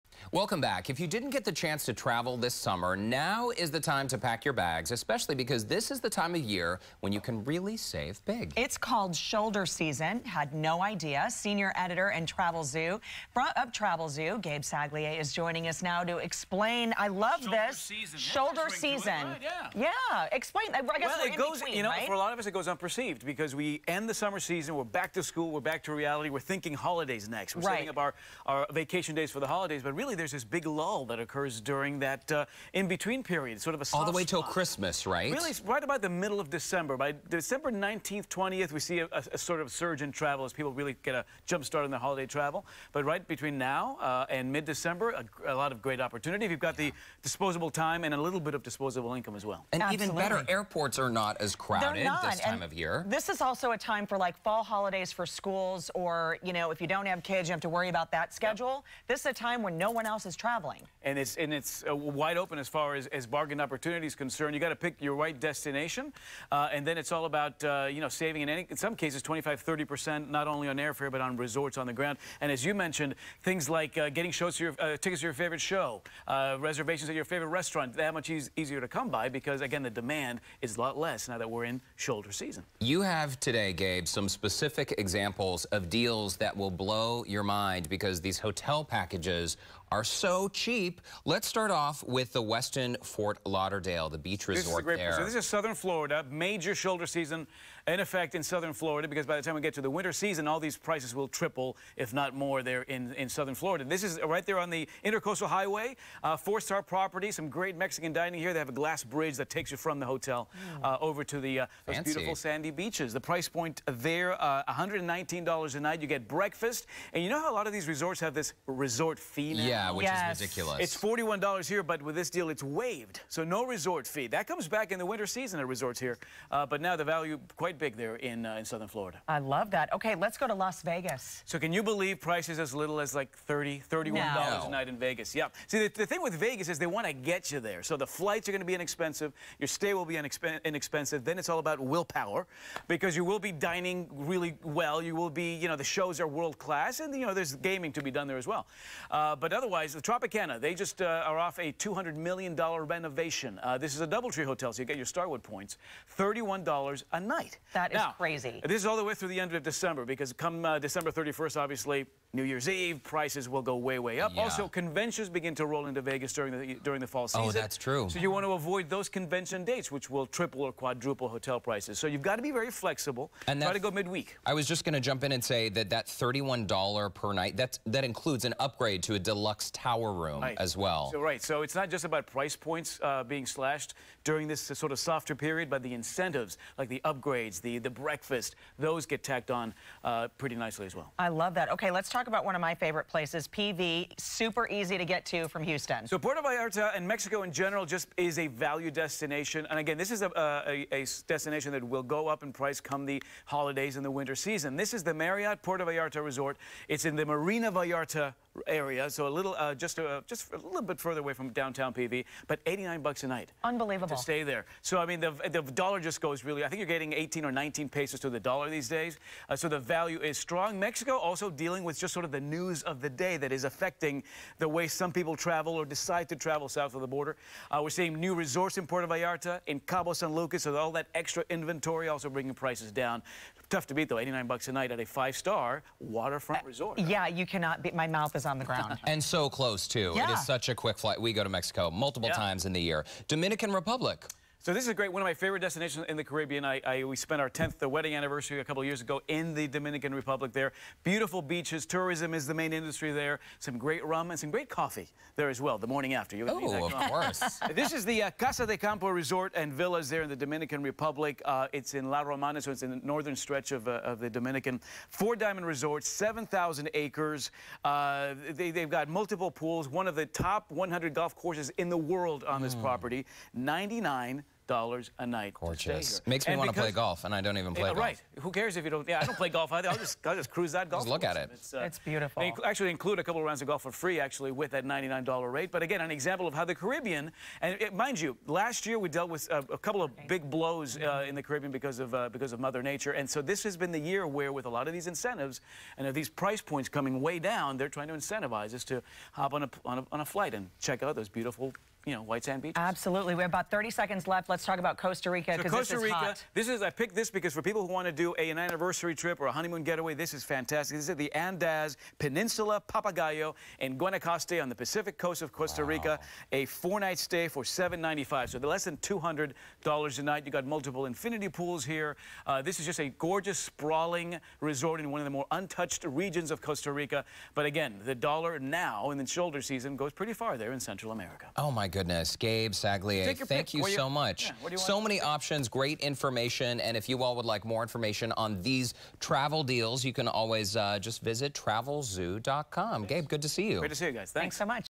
Fall Getaway Ideas
Gabe Saglie with TravelZoo shares his top picks for destinations on a budget.